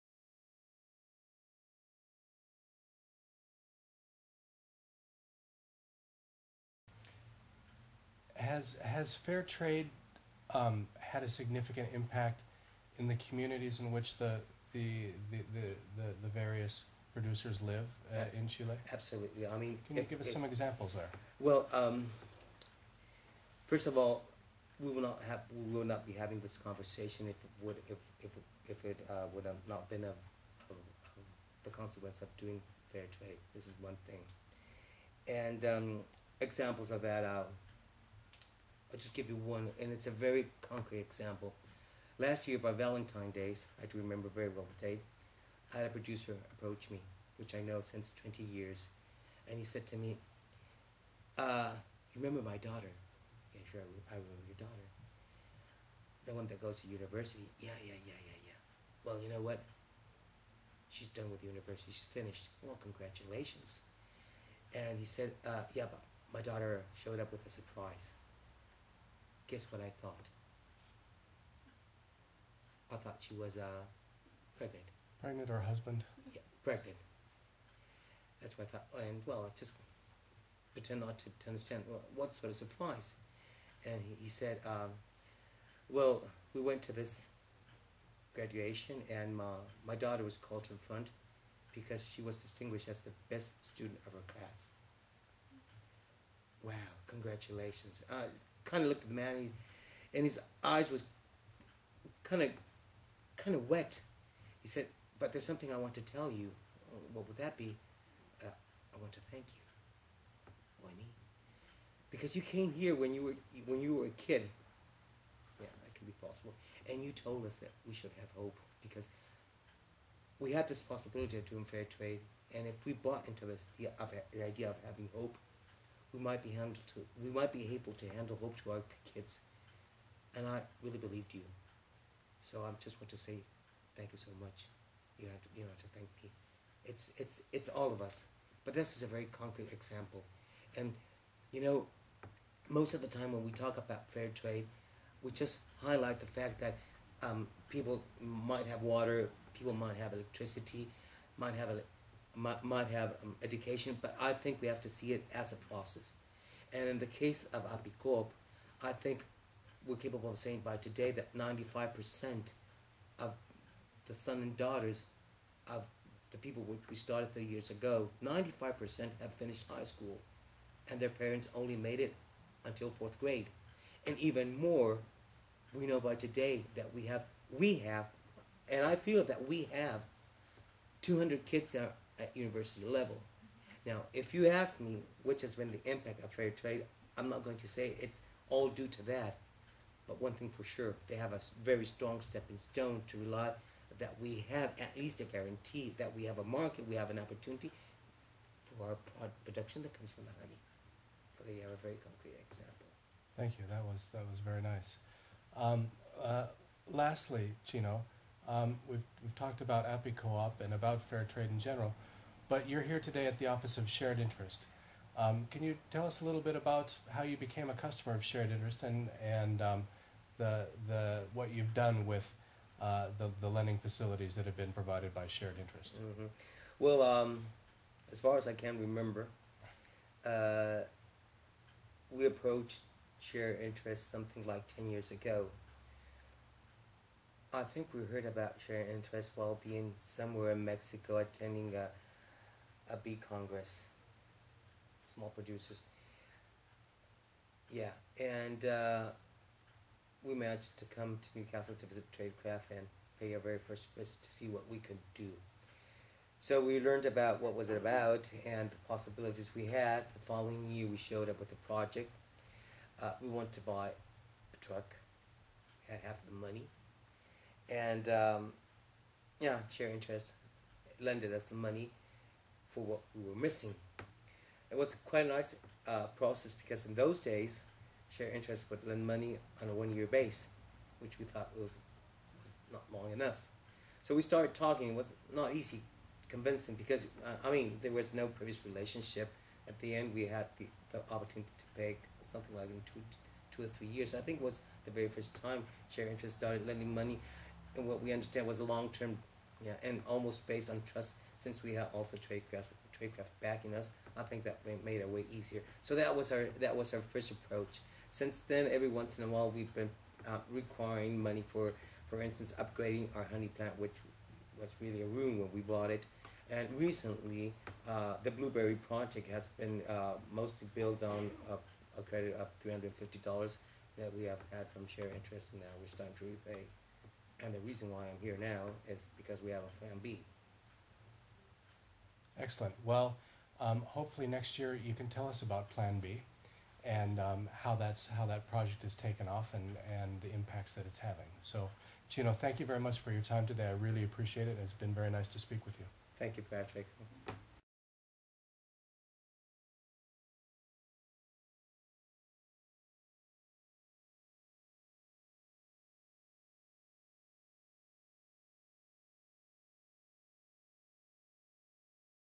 Meet Apicoop (Part Two): The Chilean Honey Producers Putting People First
Video interview with fair trade cooperative APICOOP.

In this interview, Chino the managing director of APICOOP talks about the positive impact that fair trade has had on the members of the cooperative.

Cooperativa Campesina Apicola Valdivia (APICOOP)

Ulmo flowerChile has a diverse variety of flowers of native species, herbaceous plants and trees that grow only in the central and southern areas of the country. One of these trees is Ulmo, which stands out due to its pure white flowers, with extraordinary melliferous qualities. These flowers, so abundant as to make the tree appear covered in snow, are pollinated by bees that use the nectar to produce meil de Ulmo, a speciality of northern Patagonia.

For the 120 members of Apicoop located in the Andes foothills, this is a precious natural resource that allows them to provide a sustainable income for their families. Founded in 1998, Apicoop stemmed from the Diocese Valdivia which promoted rural development.

The fact that the church was the supporting body was particularly important during the military dictatorship, when self-help groups, trade unions and farmer organisations were persecuted, and many of the activities that had, until then, been carried out by these organisations could be continued only under the patronage of the church.